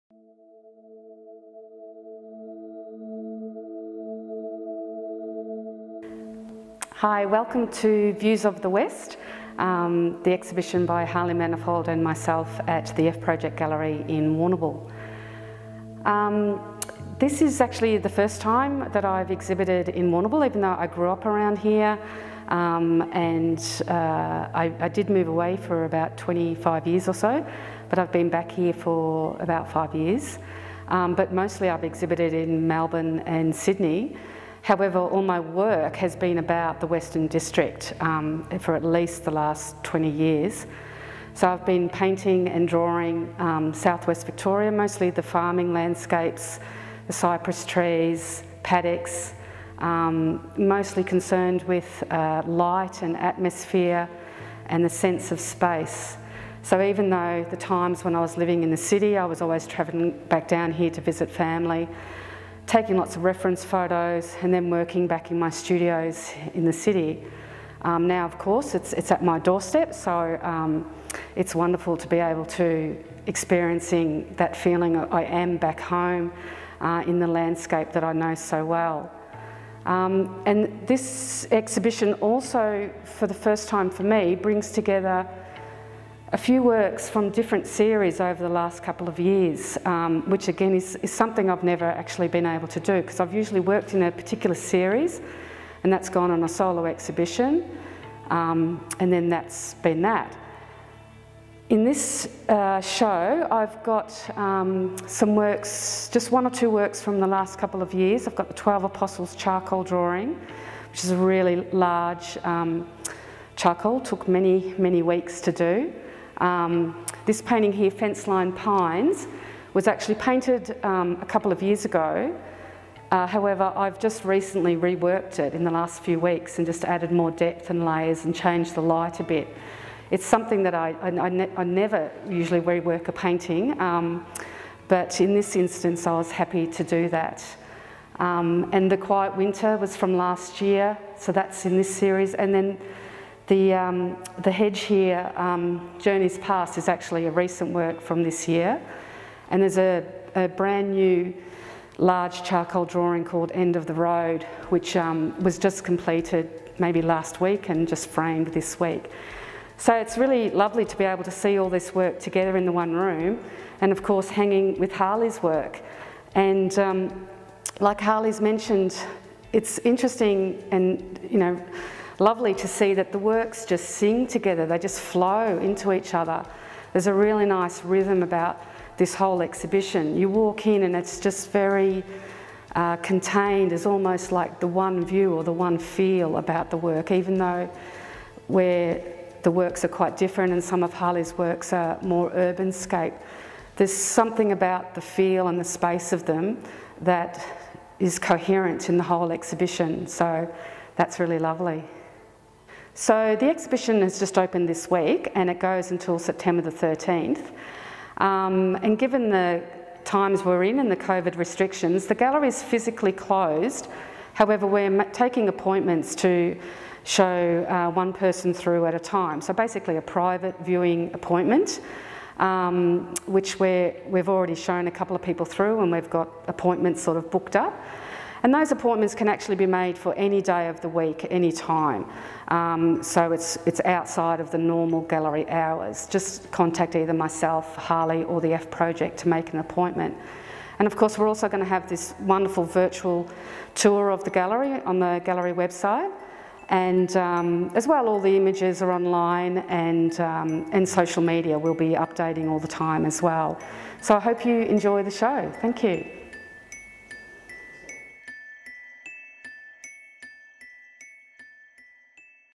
'Views from the West' Kathryn Ryan 2020
Artist Kathryn Ryan speaking about the works in our exhibition:
 Views from the West,
Kathryn Ryan & Harley Manifold
 The F Project Gallery, Warrnambool
Aug 18 - Sept 13 2020